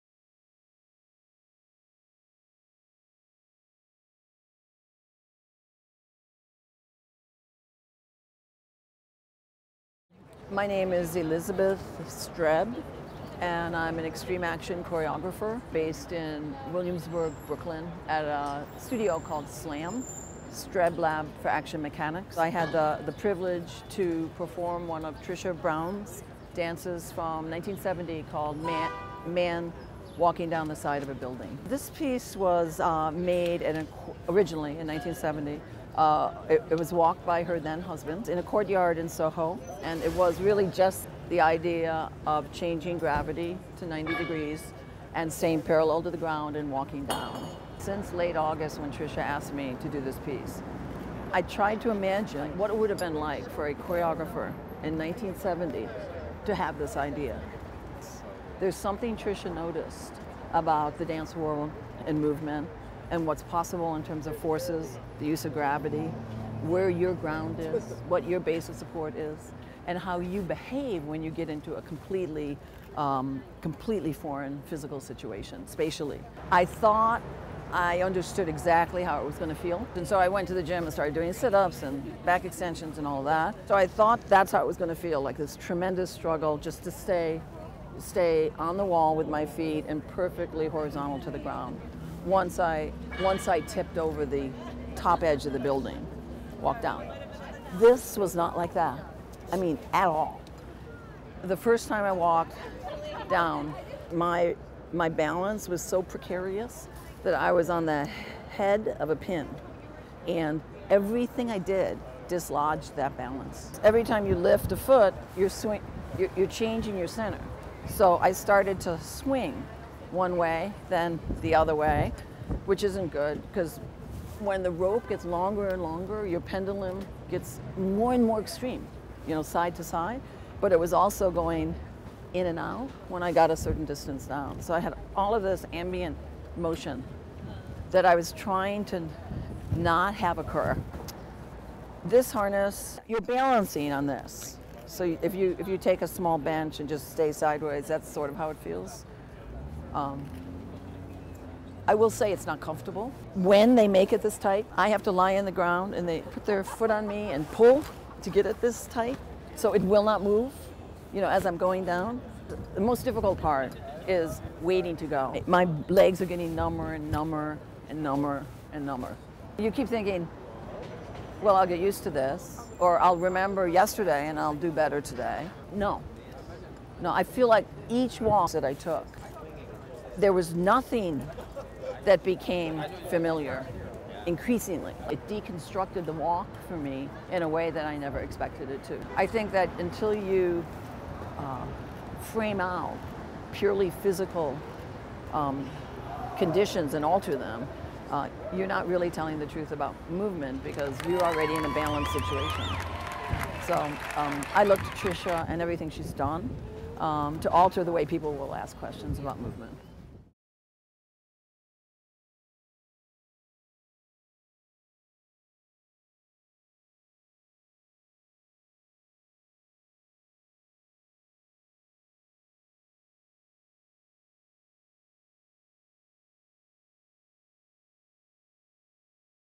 Elizabeth Streb discusses Trisha Brown's "Man Walking Down the Side of a Building"
In this video, Elizabeth Streb describes what it felt like to walk down the facade of the Museum for her performance of Trisha Brown's "Man Walking Down the Side of a Building" (1970) during "Off the Wall: Part 2—Seven Works by Trisha Brown." Streb explains how Brown's choreography often works to explore simple actions—such as walking—when taken out of their everyday context.